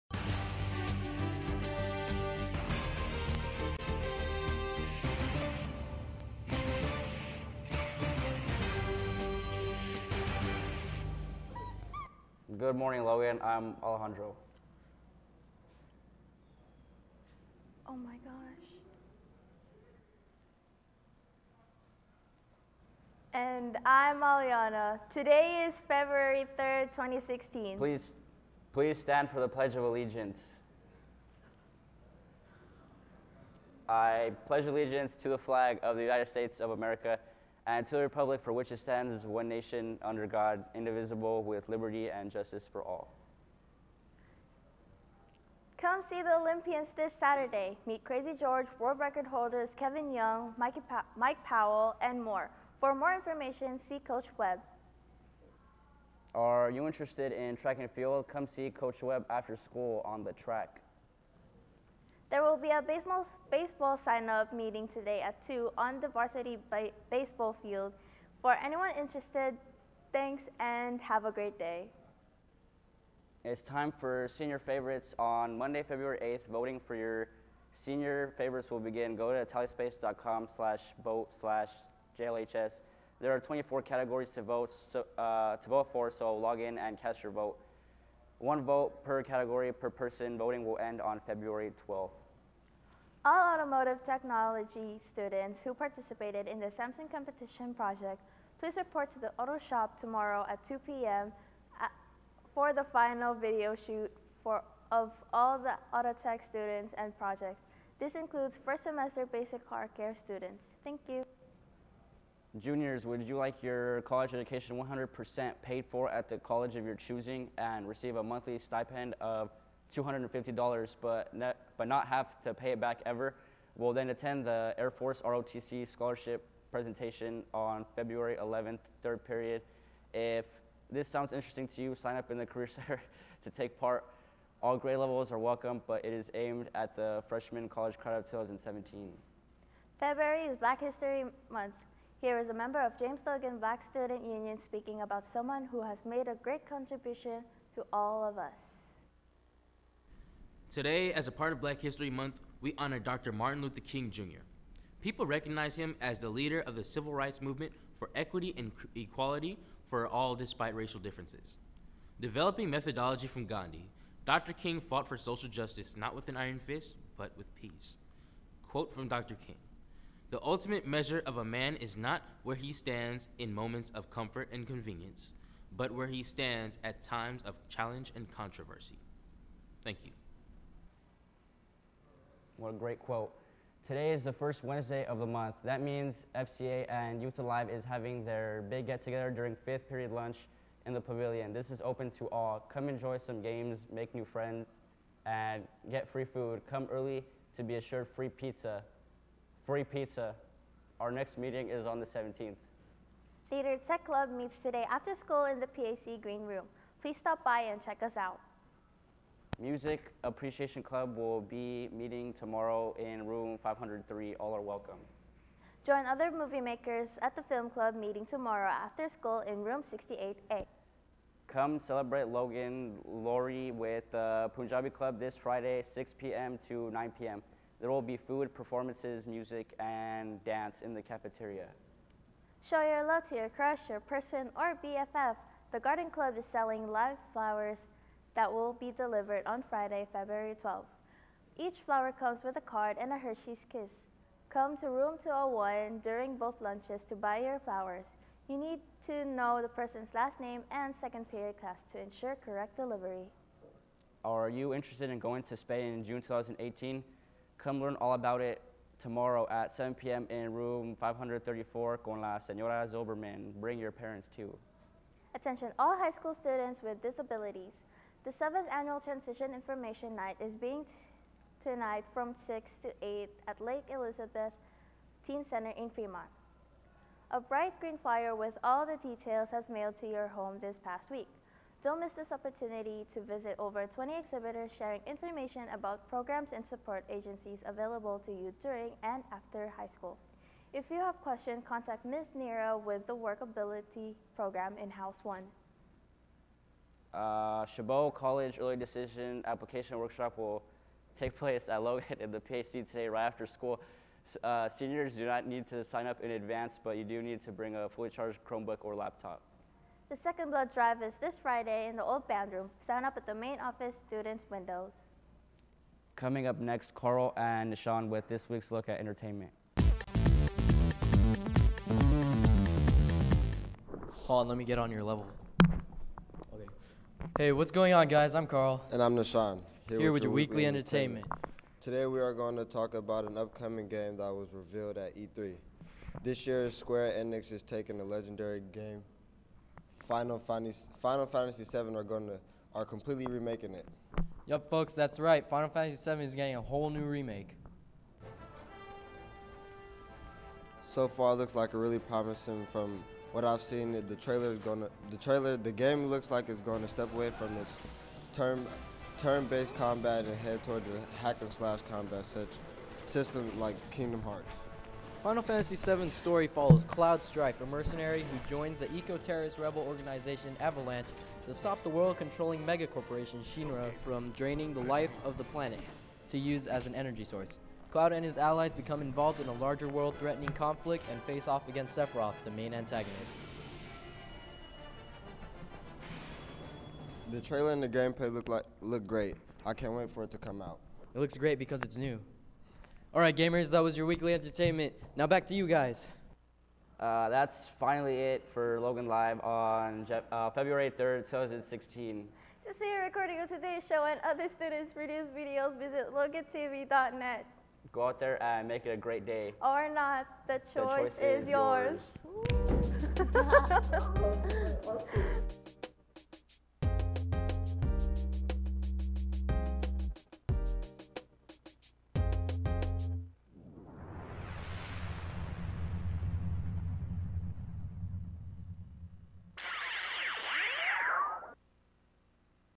Logan Live 2/3/16
Logan Live is the daily student-produced newscast from James Logan High School in Union City, California. This is the recording of the live broadcast on 2/3/16. Logan Live is broadcast most school days at the beginning of second period: 9:10AM Wednesdays, 9:25AM other weekdays.